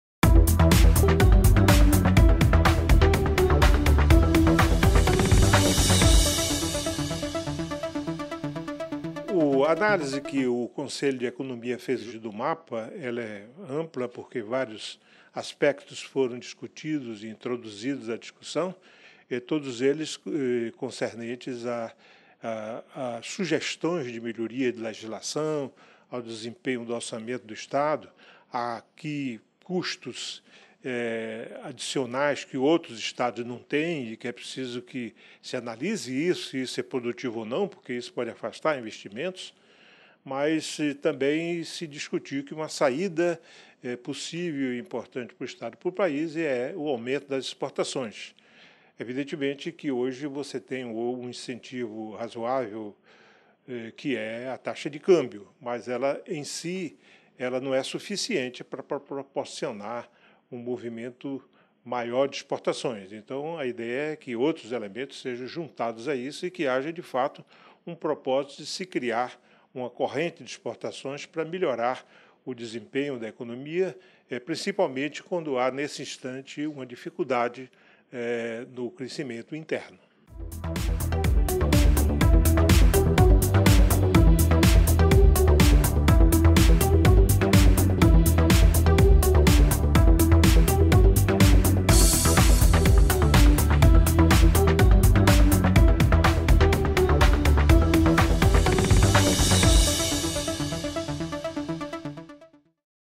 José Mascarenhas: Mapa aponta exportações como saída possível para o estado e o país
O presidente do Conselho de Economia do Sistema FIRJAN, José Mascarenhas, fala sobre a construção do Mapa do Desenvolvimento do Estado do Rio de Janeiro (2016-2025), um documento colaborativo que será criado por empresários da indústria fluminense com propostas para melhoria de questões estruturais. O Mapa servirá como bússola para a retomada do desenvolvimento sustentável do estado do Rio e do Brasil, além de nortear a atuação da Federação nos próximos 10 anos.